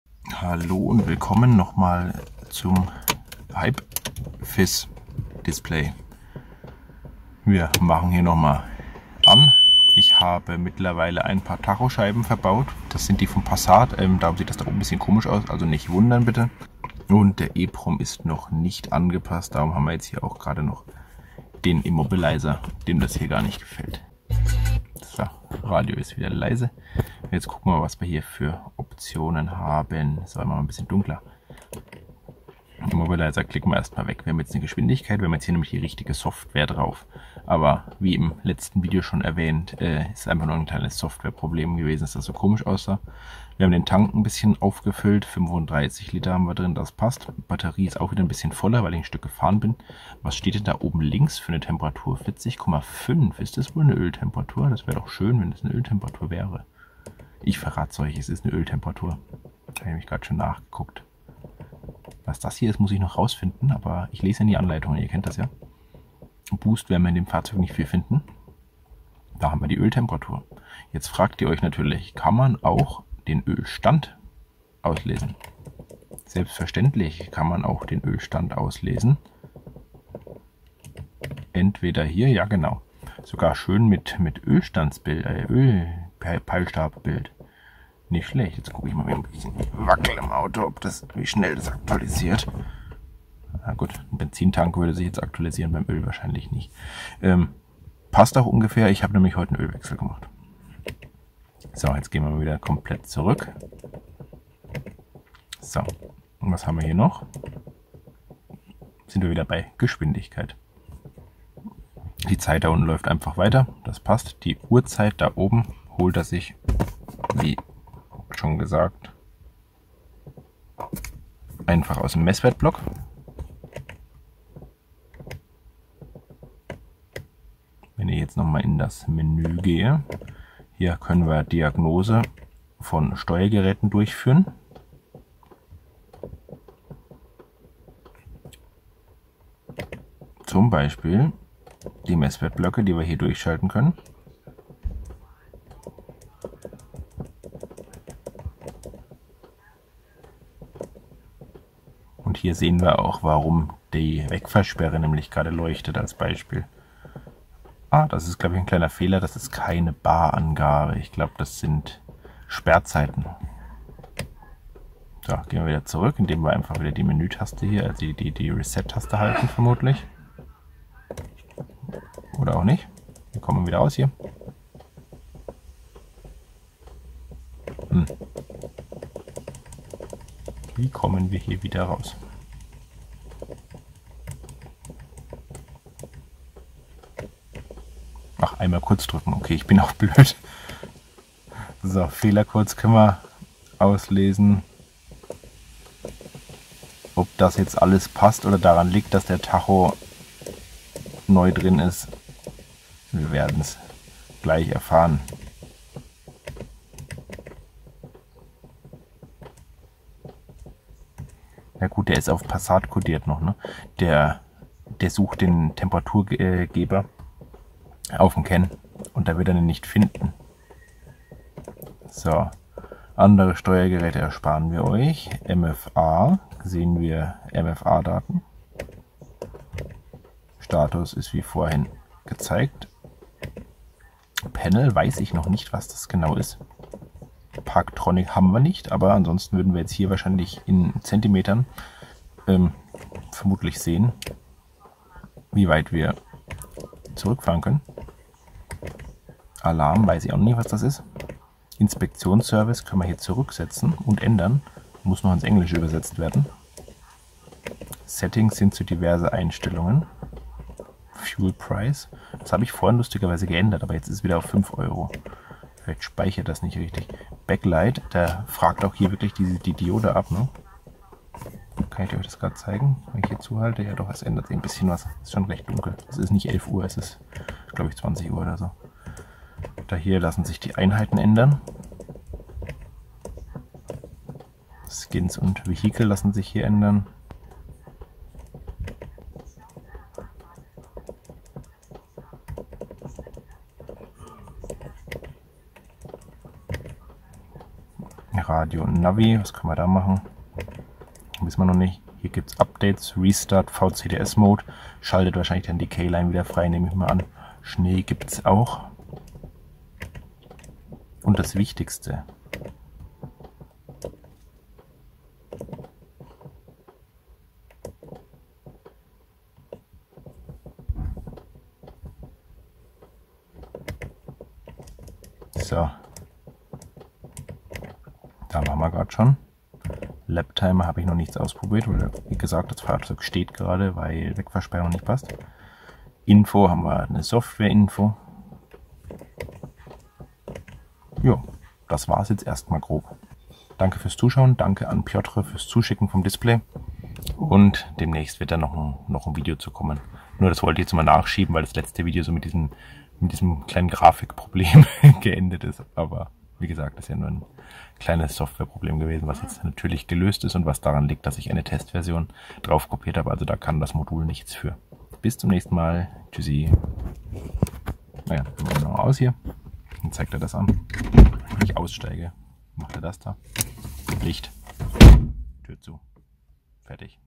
🇩🇪 VW Golf IV Passat HypeFIS Color | Teil 3 - Kurzer Funktionsüberblick
Es folgt ein kurzer Funktionsüberblick des HypeFIS Color.
Dieses Modul gibt es für 215 Euro in folgendem Shop.

Eine ausführliche Installationsanleitung steht nach Kauf zum Download bereit.

Ich übernehme keine Haftung bei Sach- und/oder Personenschäden, welche durch die Anleitung bzw. die Umsetzung entstanden sind.
Keine Garantie und keine Haftung bei Schäden, egal welcher Art, welche durch diese Anleitung entstanden sind! Jeder handelt in eigener Verantwortung.